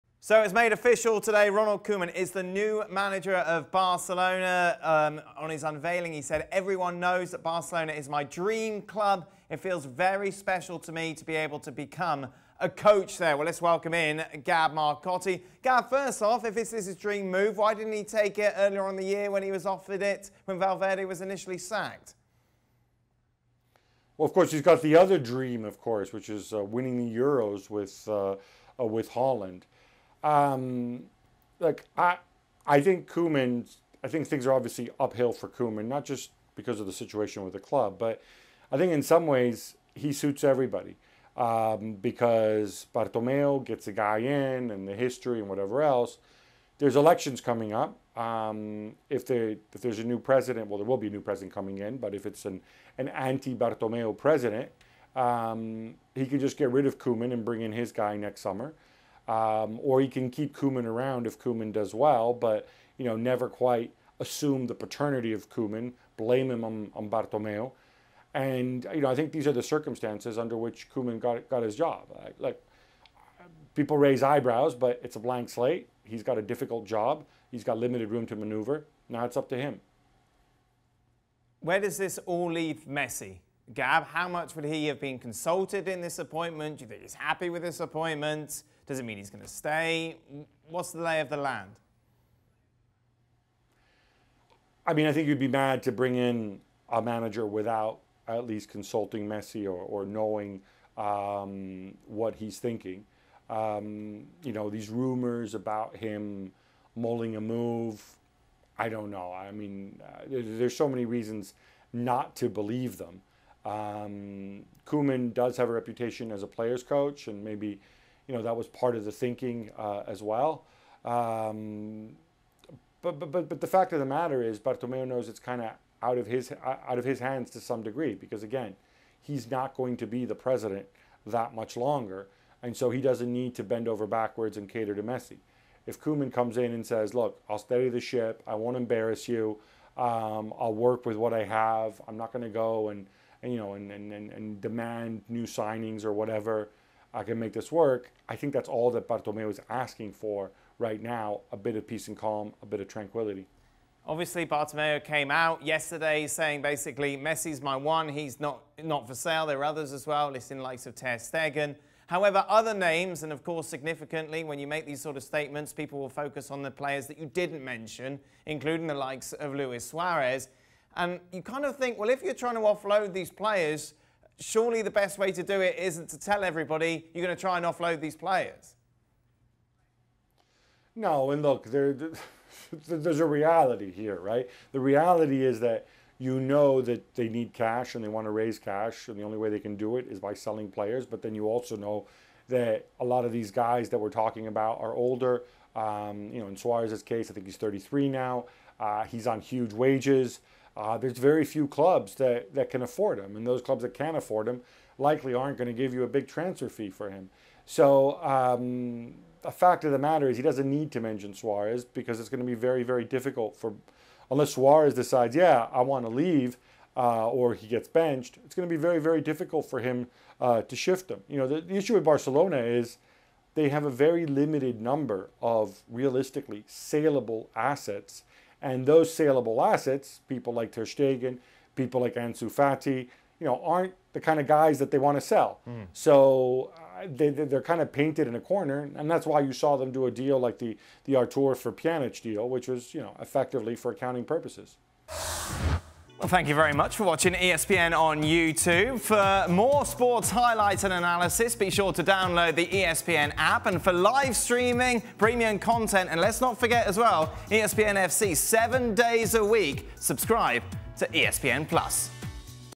Barcelona FIRE SALE: Will Ronald Koeman sell Luis Suarez, Gerard Pique & Ivan Rakitic? | ESPN FC
ESPN FC's Gab Marcotti weighs in on Ronald Koeman's appointment at Barcelona and how the new manager will proceed with rebuilding the squad. Marcotti says with president Josep Maria Bartomeu's days numbered at the club, the board will be looking for Koeman to restore "peace and tranquility." Marcotti also addresses Lionel Messi's future at the club and questions how many players Barca can realistically sell given their high wages.